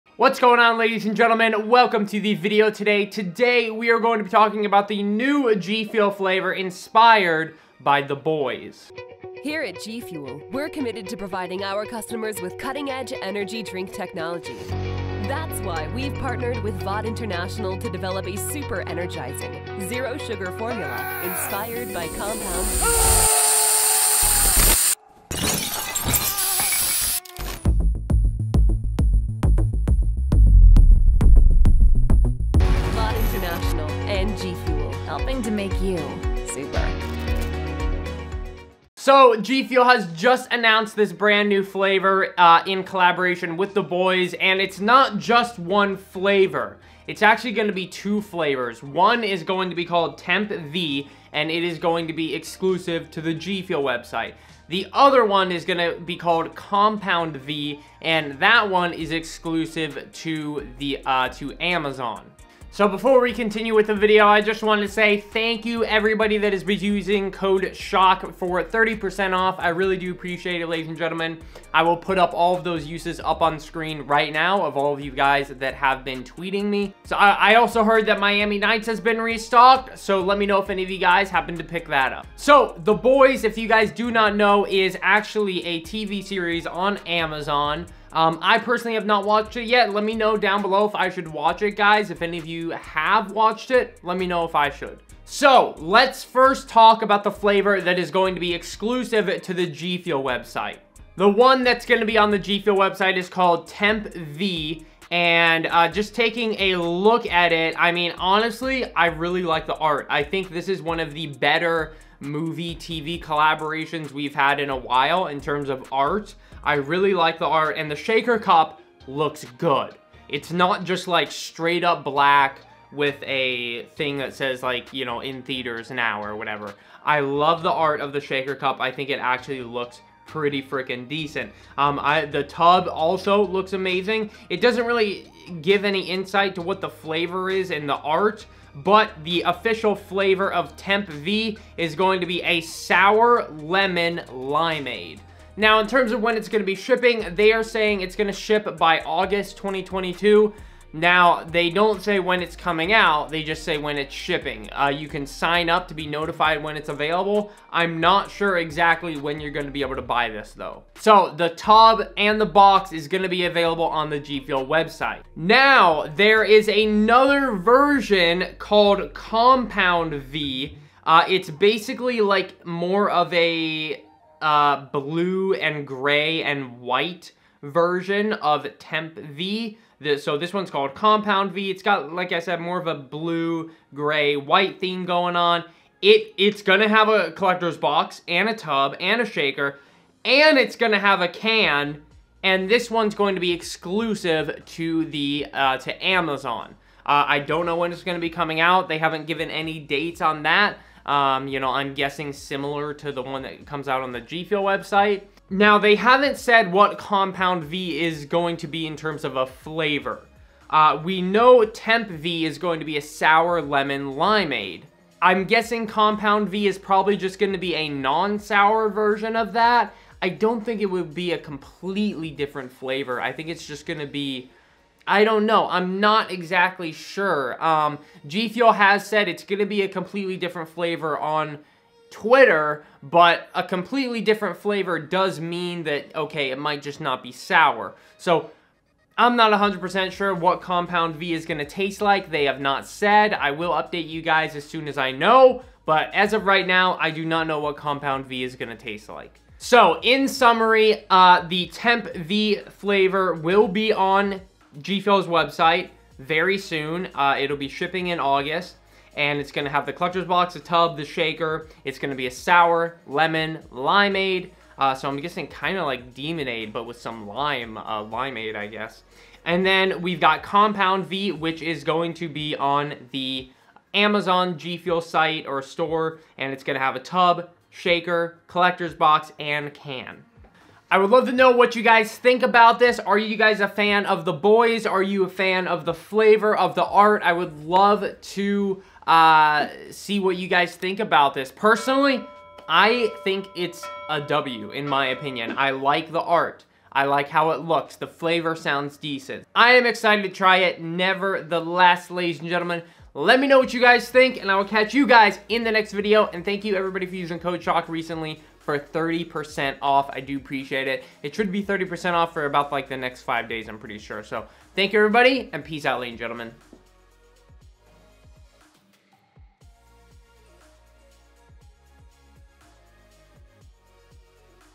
The Boys GFuel Flavor! - Temp V & Compound V!
Ladies and gents today I wanted give you guys all the info you need on the NEWEST GFUEL flavor inspired by the TV Series "The Boys"

►► SETUP GEAR

Reid/Shock
Merrimack NH
03054